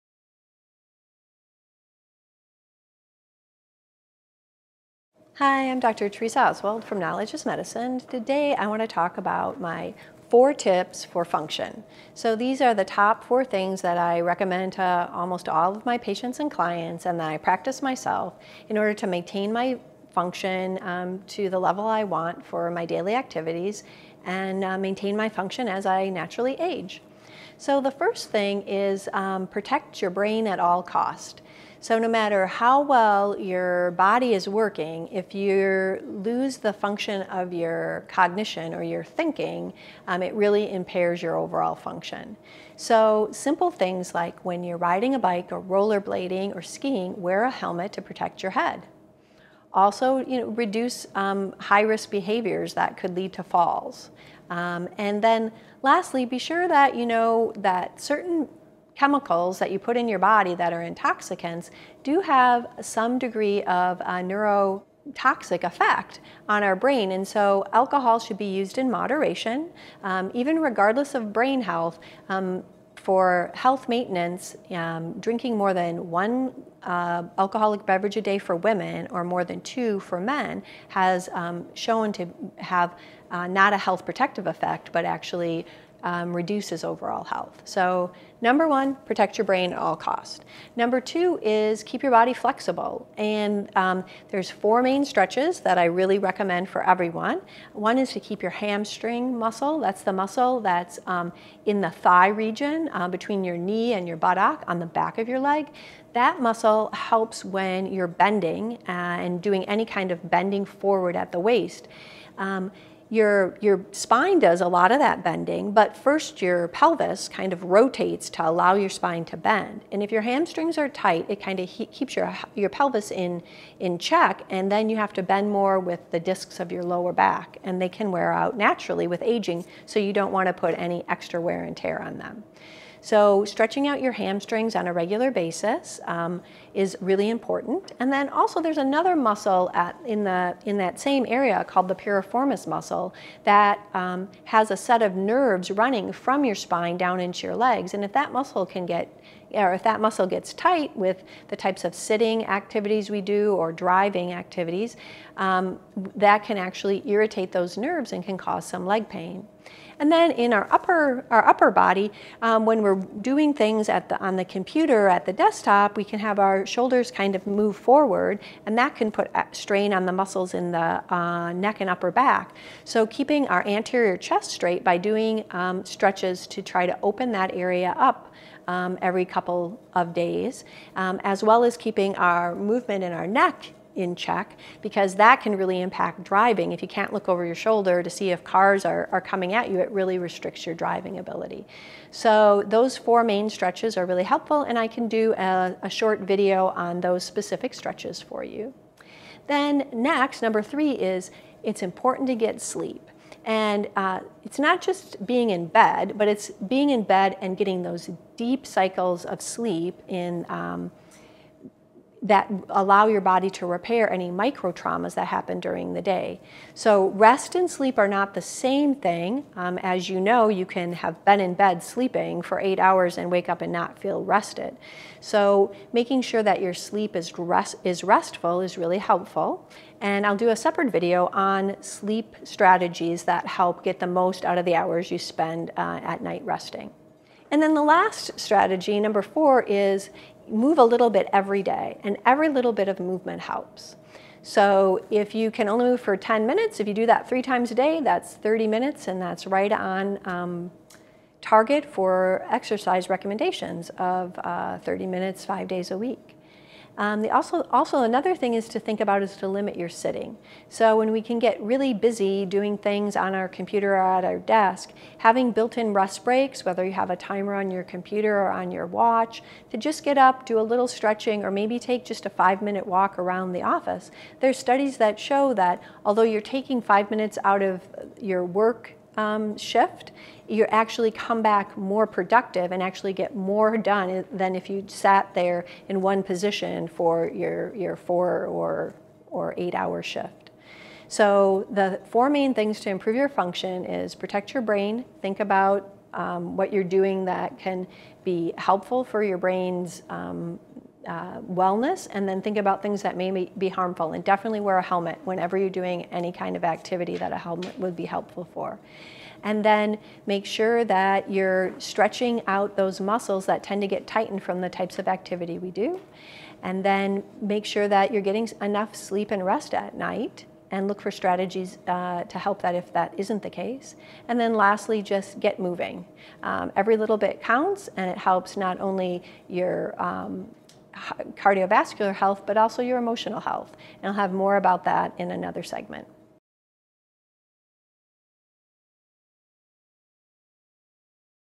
4 Tips for Function
Dr. Theresa Oswald gives 4 tips to keep up your function as you grow into your adult years.